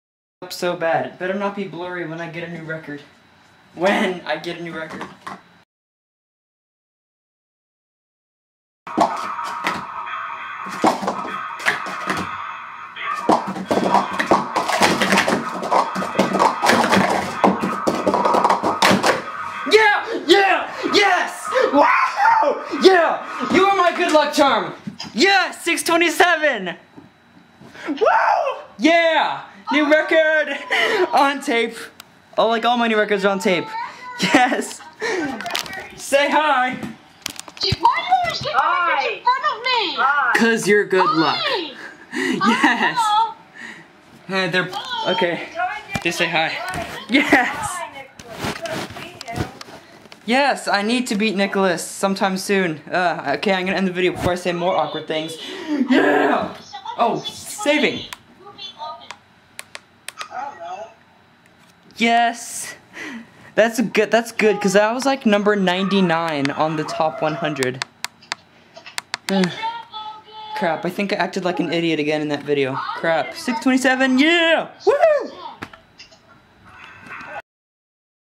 Super Blurry NEW RECORD 6.27!!! SO BLURRY!!
this is blurry. eh i think i can go faster so i will try to get another new record soon in a less blurry spot. i think its cuz the bright green wall and it cant focus right... ugh BUT YES NEW RECORD

shoutout to crackerstacker96 for being good luck... i was chatting with josh, my old good luck charm for an hour and then he left and then dalton called and 1 minute 43 seconds later i got this blurry time!